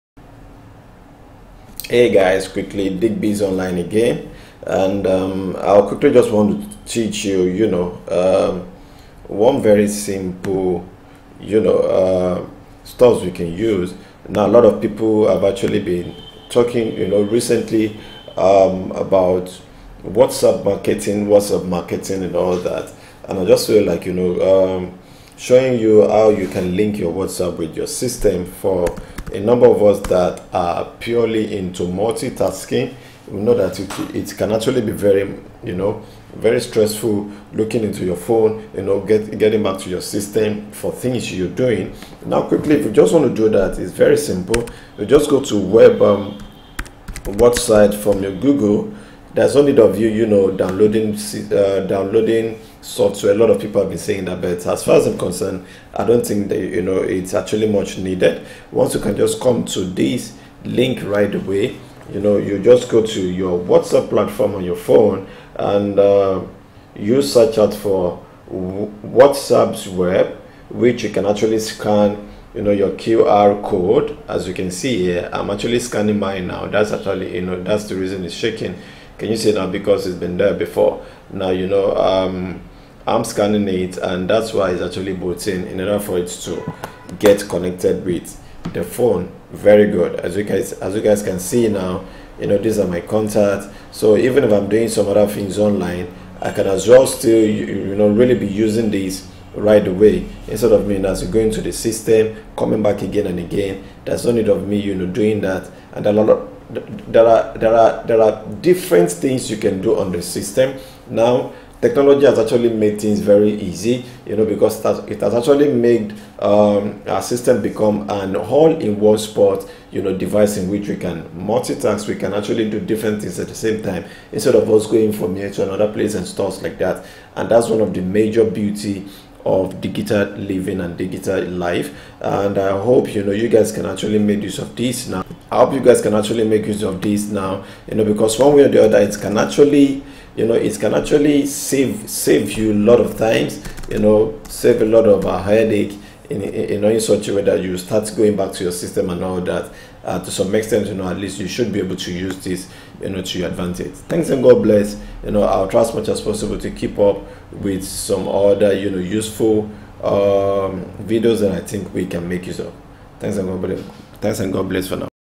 How to Connect Whatsapp to your Laptop
in this video i showed you how to connect your whatsapp with your system in order to reduce the workload among your different device

           thanks